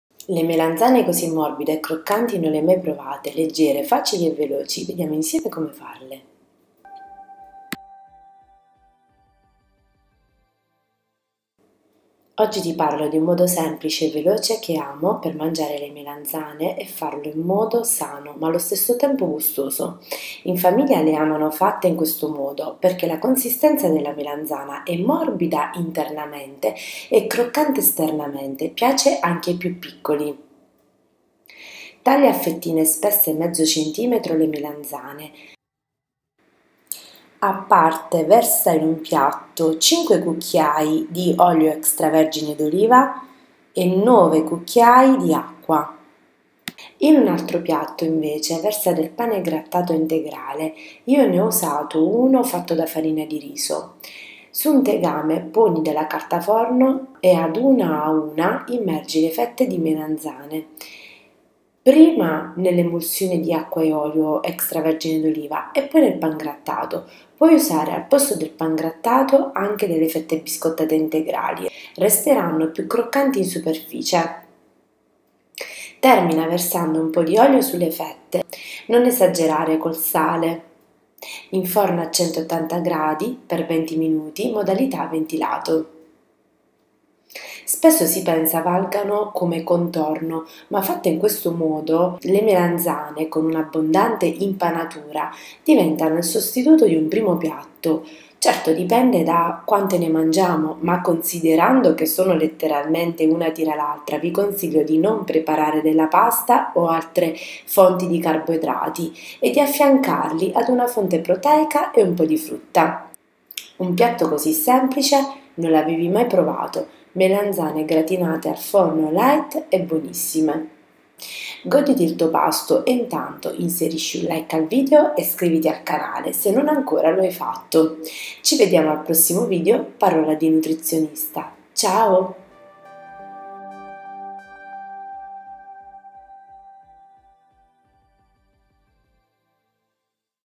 Melanzane gratinate al forno, light e buonissime
Le melanzane così morbide e croccanti non le hai mai provate, leggere, facili e veloci.
Oggi ti parlo di un modo semplice e veloce che amo per mangiare le melanzane e farlo in modo sano ma allo stesso tempo gustoso.
In famiglia le amano fatte in questo modo, perché la consistenza della melanzana è morbida internamente e croccante esternamente, piace anche ai piu’ piccoli.
Spesso si pensa valgano come contorno ma fatte in questo modo, con un'abbondante strato di impanatura diventano il sostituto di un primo piatto, certo dipende da quante ne mangiamo, ma considerando che sono letteralmente una tira l’altra vi consiglio di non preparare della pasta o altre pietanze ricche in carboidrati e di affiancarli ad una fonte proteica e un po’ di frutta.

Ci vediamo a prossimo video,
parola di Nutrizionista,
Ciaooo